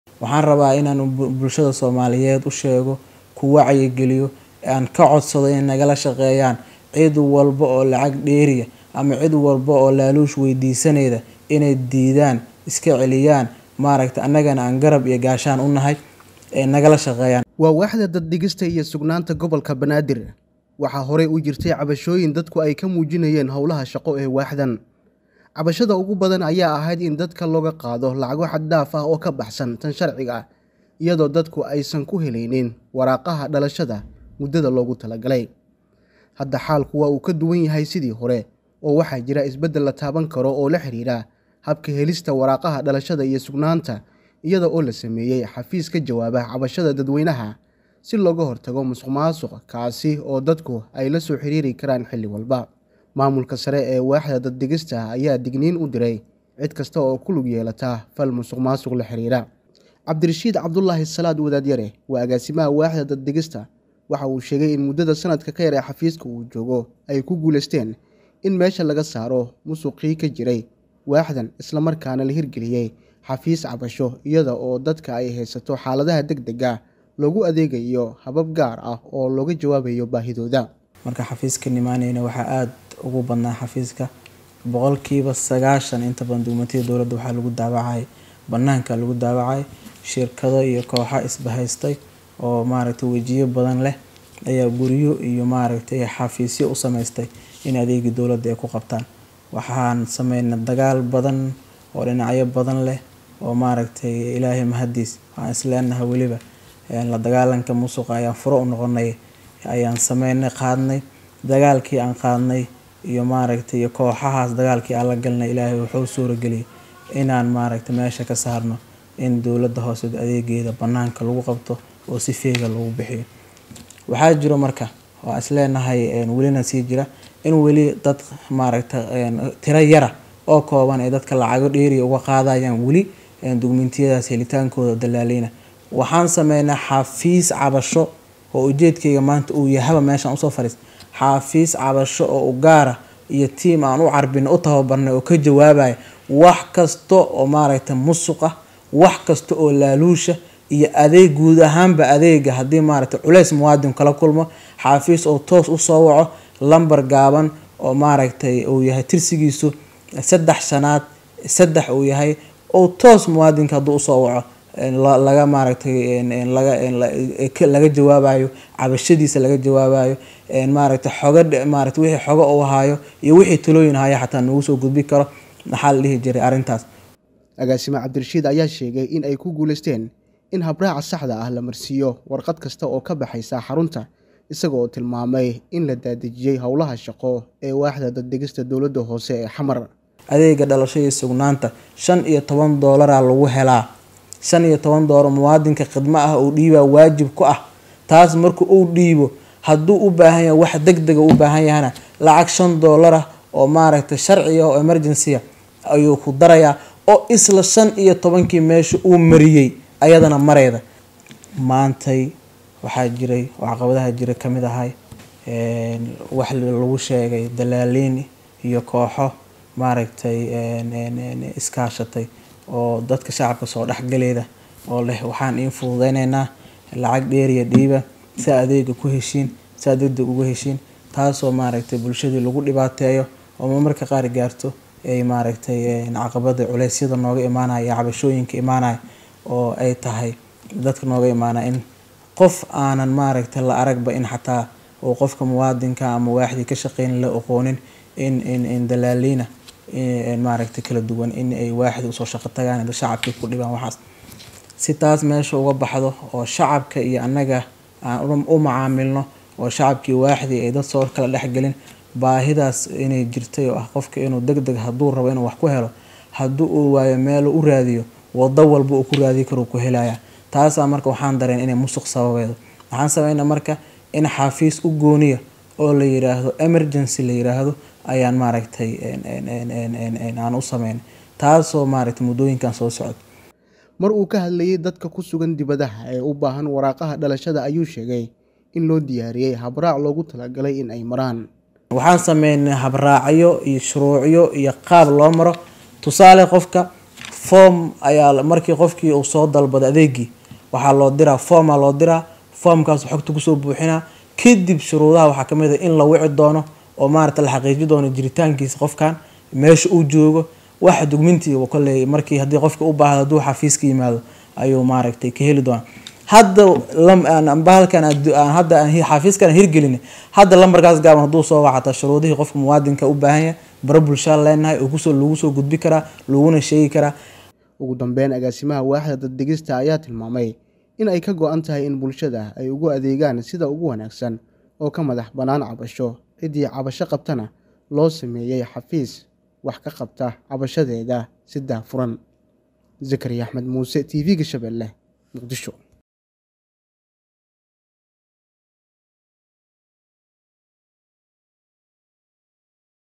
Maamulka Gobolka Banaadir oo shaqaalihiisa uga digay musuqmaasuqa, bulshaddana ka ..................
Maamulka Gobolka Banaadir oo shaqaalihiisa uga digay musuqmaasuqa, bulshaddana ka codsaday in cidii laaluush weydiisa ay lasoo wadaagaan .